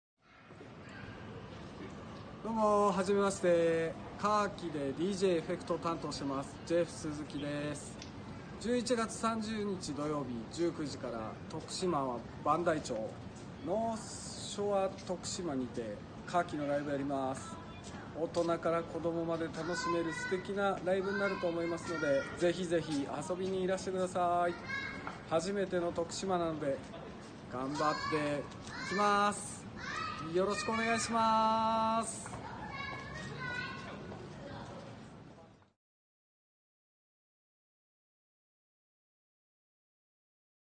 Khaki 徳島 LIVE 2019/11/3O【告知動画】Jeff Suzuki ミキサー持ち上げ君
Notre bel abri fait avec de la musique et des biens divers
『音楽と雑貨で作られた美しいシェルター』

「Khaki」
TOKUSHIMA  LIVE
2019/11/3O（sat)

19:OO
open
19:3O
start

会場
northshore

3OOO円 ADV
35OO円 DAY
ワンドリンク
別途5OO円

小学生　無料
中学生　1000yen
高校生　半額

オープニングアクト
LIVE
gojeanpaul

DJ
frawar
shinnosuke
&more

goods ◎ツバメとあじさい
雑貨
◎atelier_ortensia
ドライフラワー
◎bem
アクセサリー
◎fulful
アロマハーブ
&more

チケット
取り扱い店舗

PONT NEUF
atelier_ortensia
ツバメとあじさい

お問合せ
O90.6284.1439
（12:OO-19:OO）

メール返信が5日経っても
届かない場合はお手数ですが
直接お電話下さい

 「Khaki」
kujun （俳優 浅野忠信実兄）率いる
サウンドスケープ型記憶想起
チルビエント・ロック・バンド